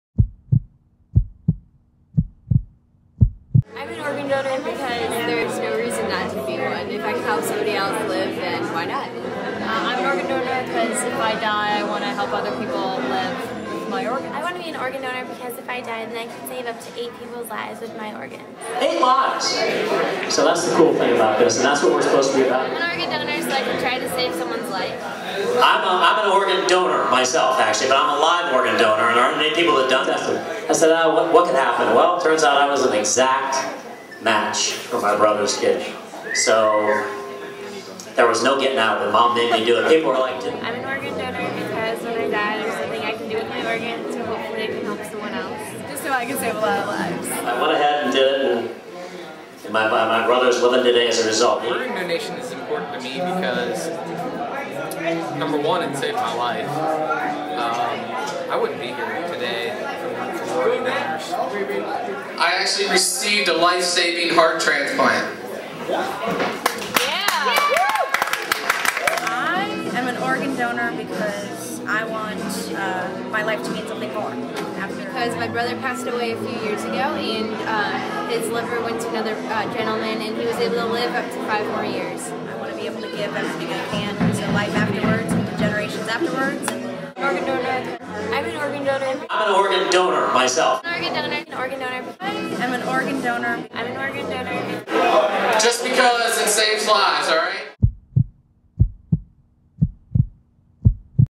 Life Connection of Ohio with University of Dayton
This video is from a comedy night event that the University of Dayton student group, Students for Organ Donation, held for Life Connection of Ohio. A lot of laughs were had and a lot of awareness was raised.